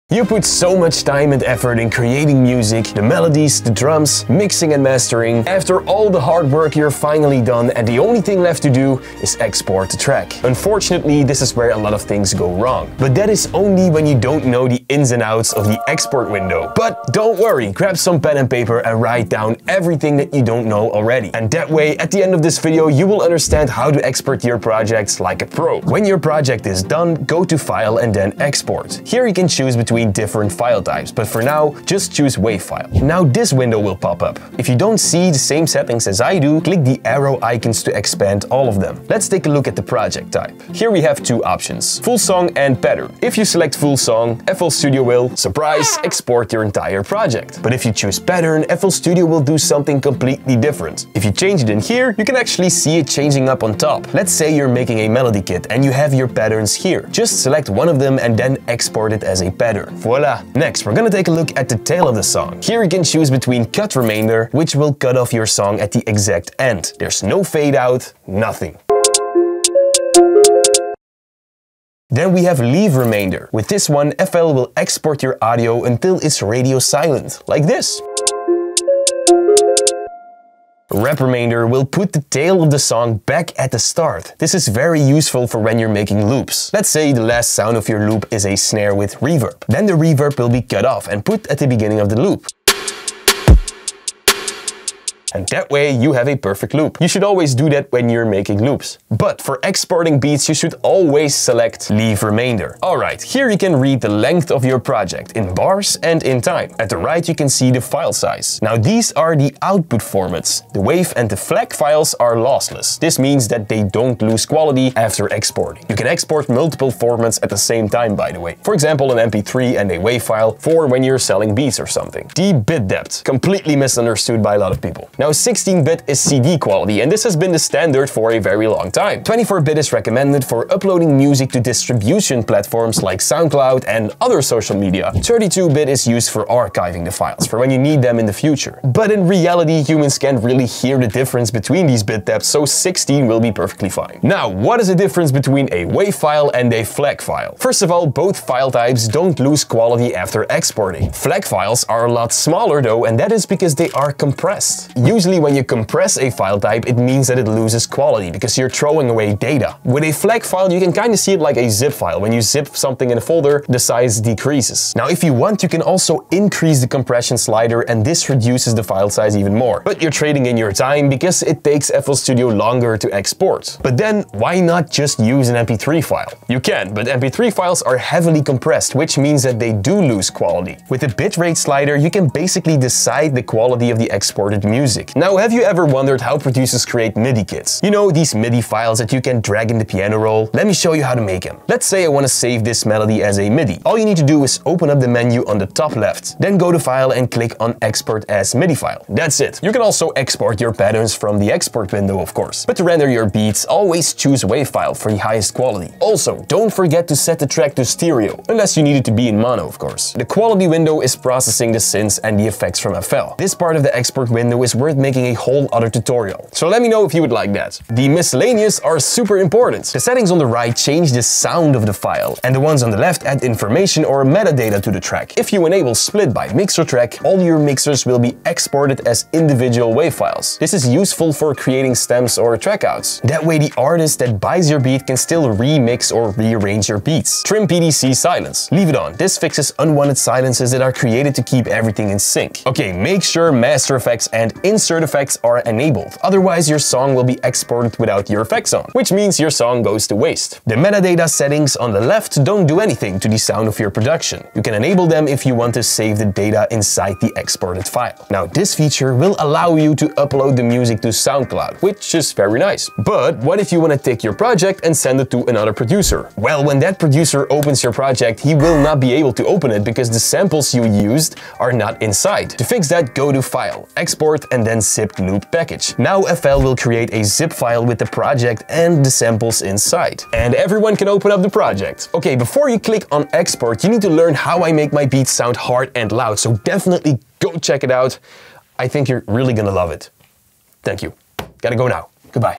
The BEST EXPORT Settings For Beats (FL Studio 21)
👉 Free 374+ FL Studio Secrets Videos In this FL Studio 21 tutorial, you will learn the in and outs of the export window. Learn how to export your beats at high quality.